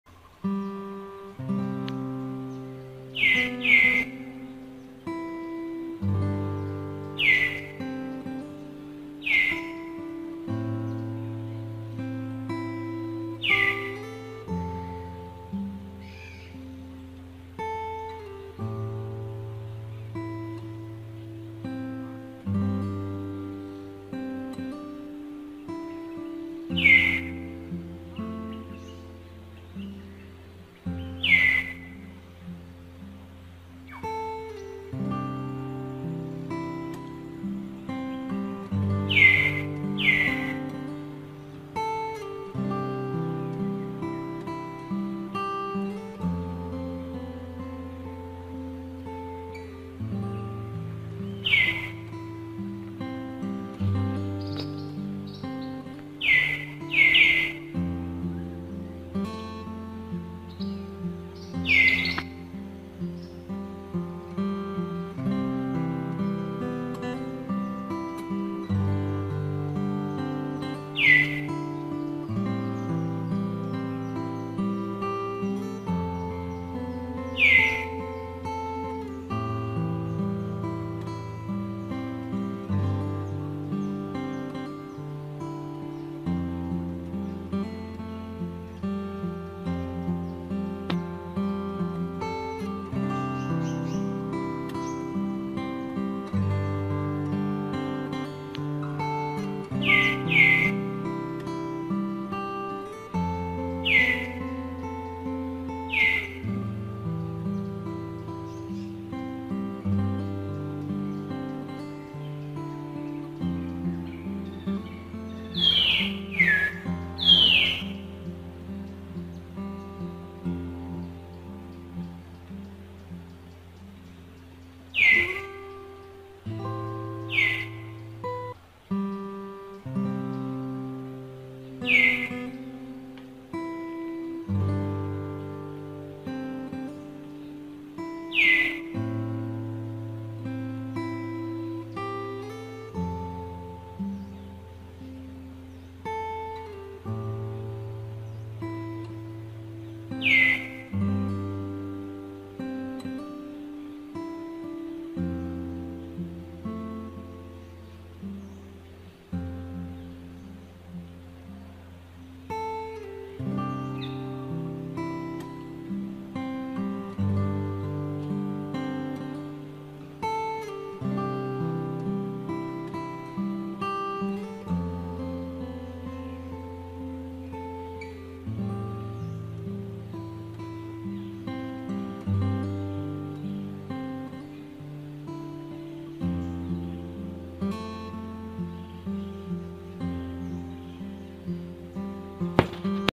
RaJo want to catch Rainbow from Libby, Libby don allow do like this
Welcome to watching my channel Dom navy. we will update monkeys in Cambodia, Both short-tailed monkeys and Long- tailed with Adorable baby.